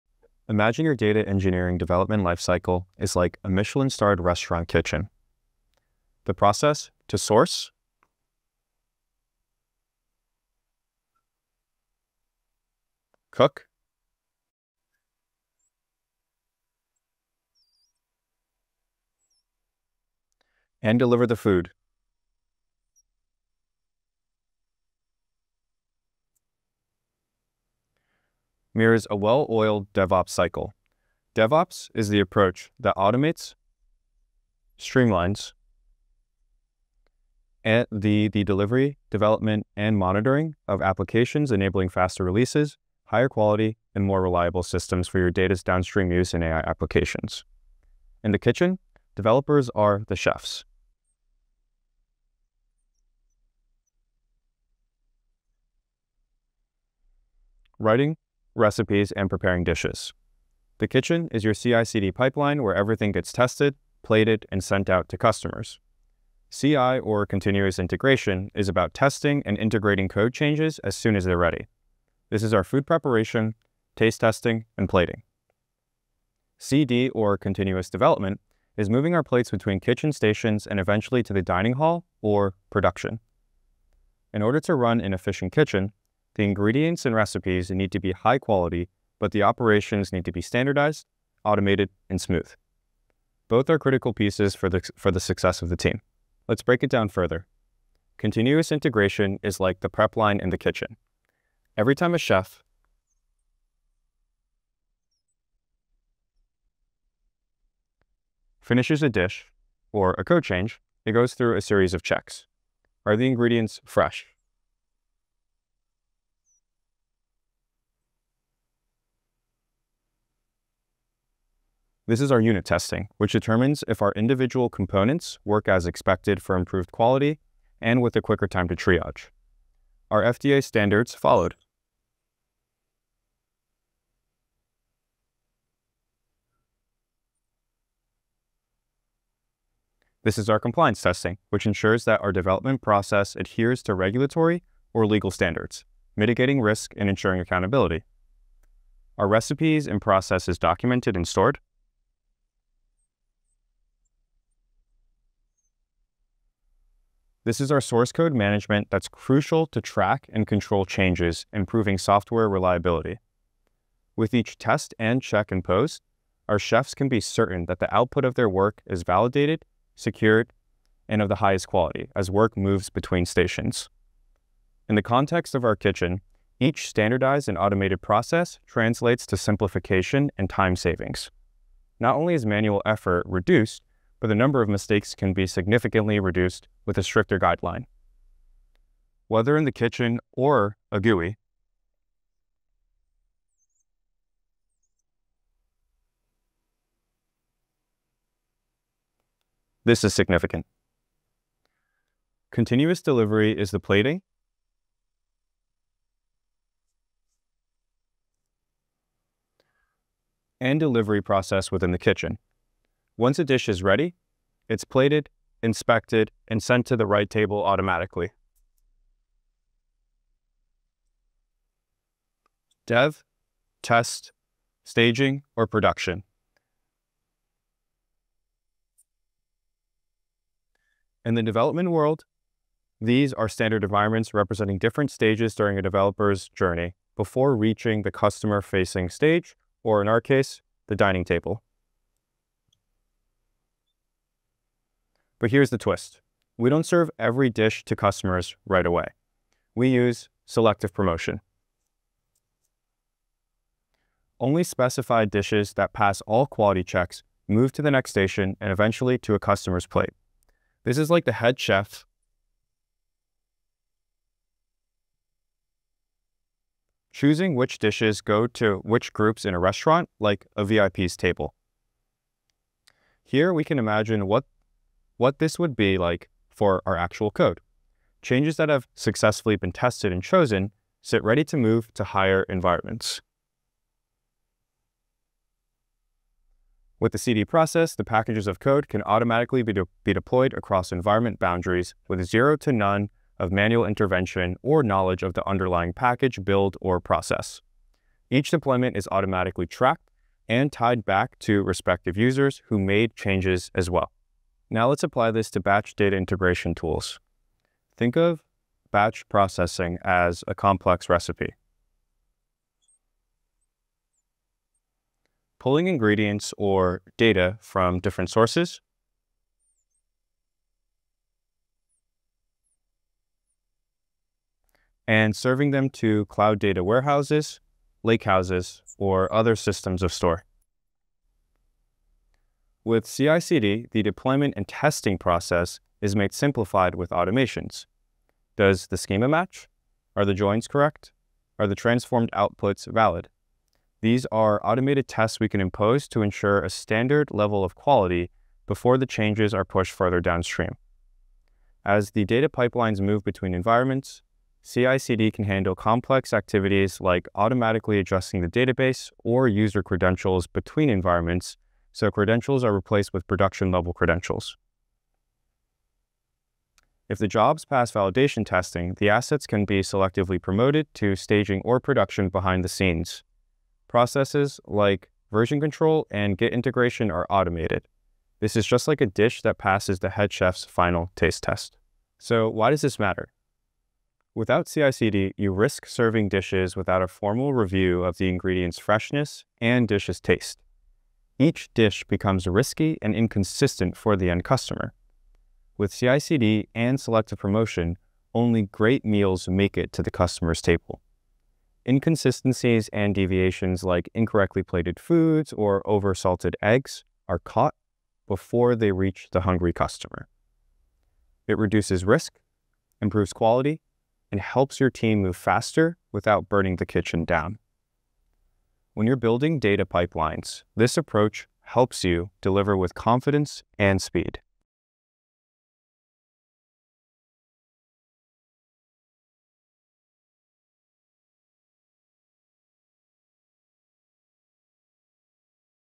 DevOps for Data Engineering: Streamline CI/CD for AI & Data Pipelines
Ready to become a certified watsonx Mainframe Modernization Architect?

Imagine your data engineering workflow as a Michelin-starred kitchen! 🍽️ Adrian Lee shows how DevOps and CI/CD pipelines boost automation, batch processing, and data pipeline reliability—helping you streamline workflows, optimize pipelines, and deliver quality at scale.

Sign up for a monthly newsletter for Cloud updates from IBM